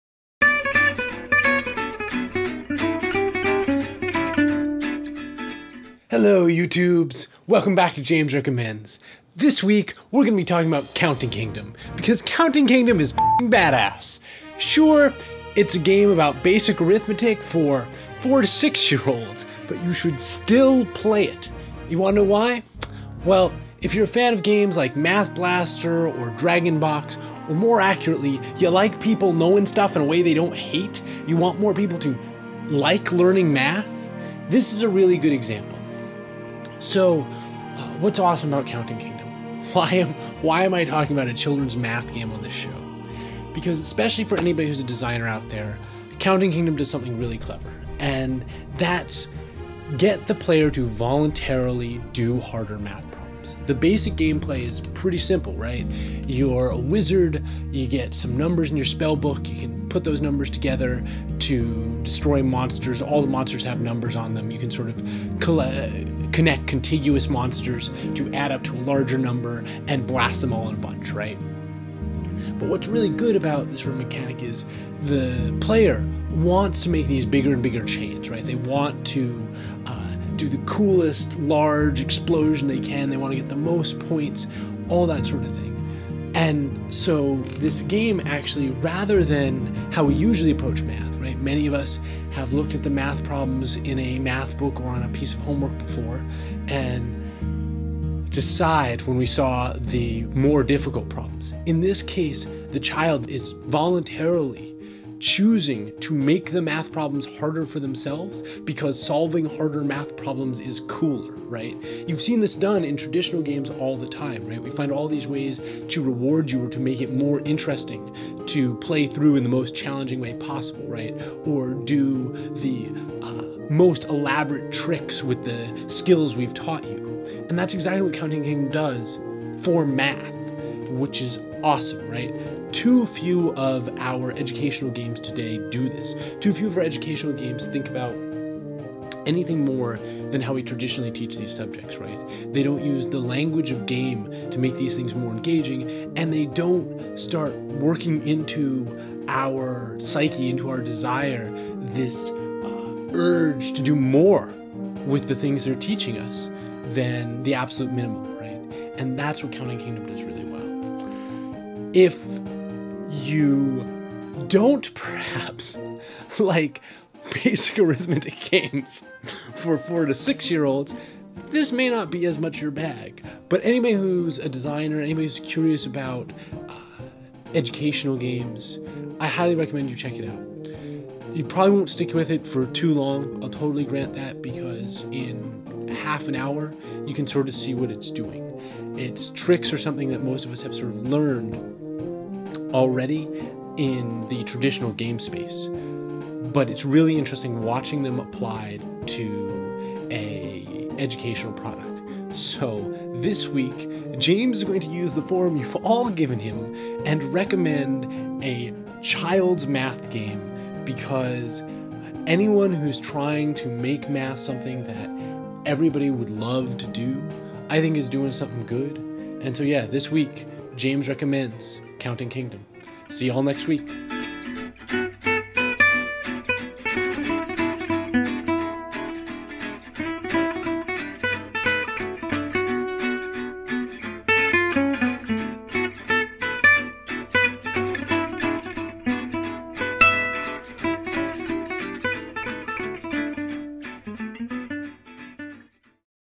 James Recommends - Counting Kingdom - Want a Good Educational Math Game?
Platform: PC, Mac, iOS (Fall 2014)

James Recommends Counting Kingdom, an educational game that introduces children ages 4 through 6 to the basic principles of math (mainly addition and subtraction). The basic tower defense mechanic teaches kids to use their math skills to defeat monsters, while top scores and rewards encourage them to seek out more difficult problem sets for themselves and challenge the limits of their mathematical knowledge.

Check for hidden annotations in the video or turn on subtitles for Bonus Facts about The Counting Kingdom!